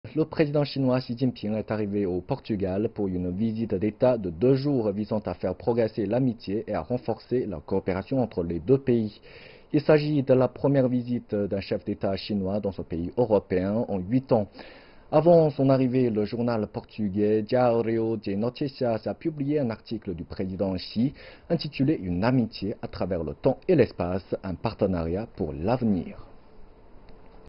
Le président chinois arrive à Lisbonne pour une visite de deux jours
Le président chinois XiJinping est arrivé au Portugal pour une visite d'État de deux jours visant à faire progresser l'amitié et à renforcer la coopération entre les deux pays. Il s'agit de la première visite d'un chef d'État chinois dans ce pays européen en huit ans. Avant son arrivée, le journal portugais Diario de Noticias a publié un article du président Xi intitulé "Une amitié à travers le temps et l'espace, un partenariat pour l'avenir".